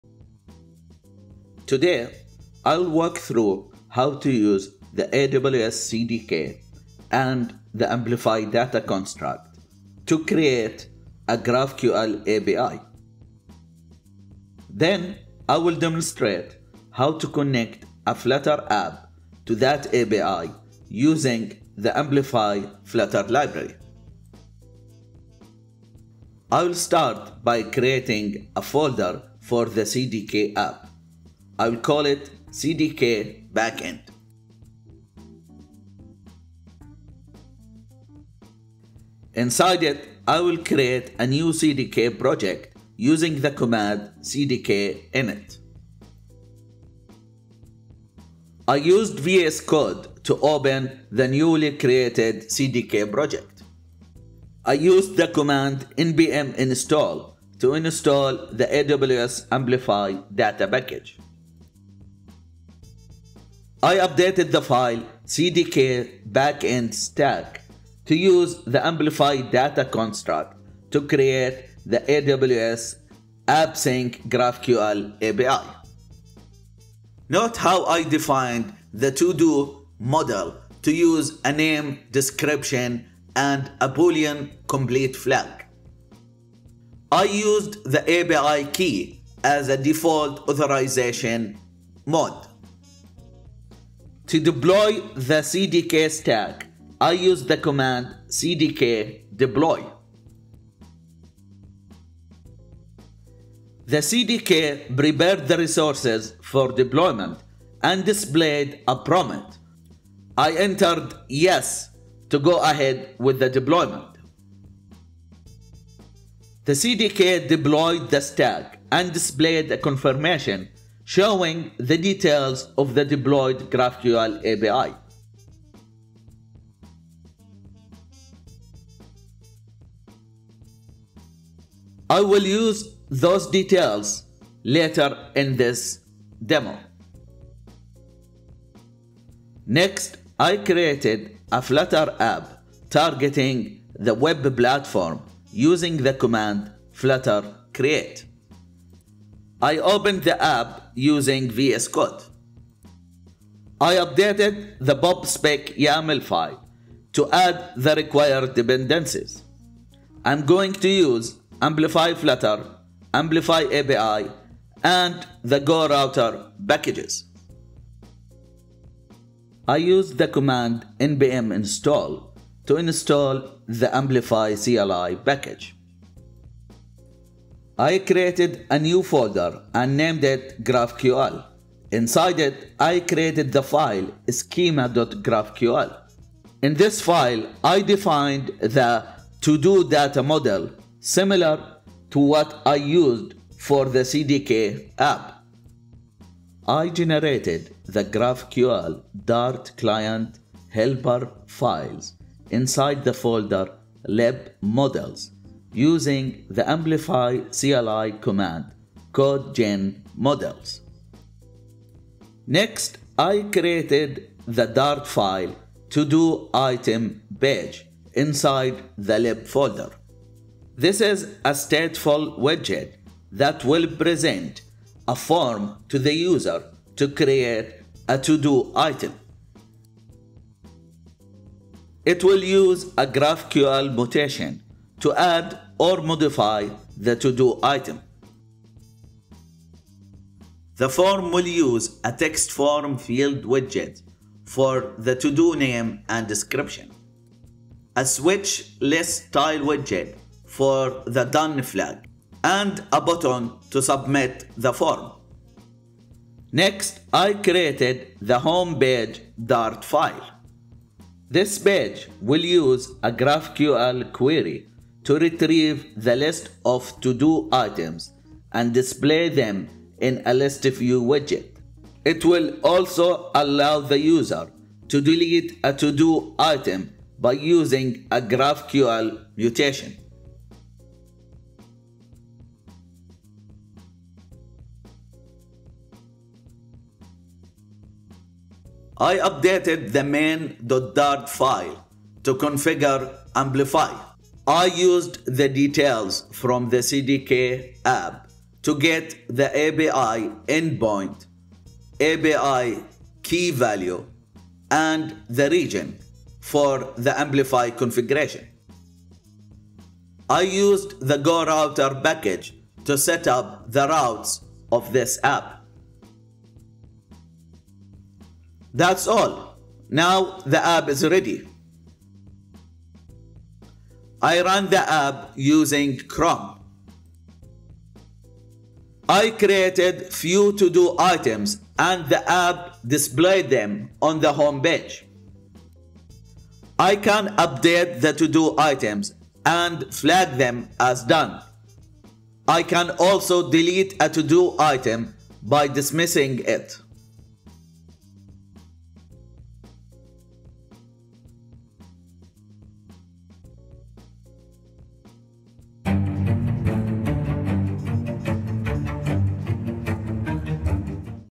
Flutter & GraphQL: Connecting a Flutter App to an AWS AppSync API Built with CDK
Learn how to use the AWS Cloud Development Kit (AWS CDK) and the Amplify Data construct, recently launched by AWS Amplify, to define a GraphQL API backend using AWS AppSync. Then connect a Flutter application to that API using the Amplify Flutter library.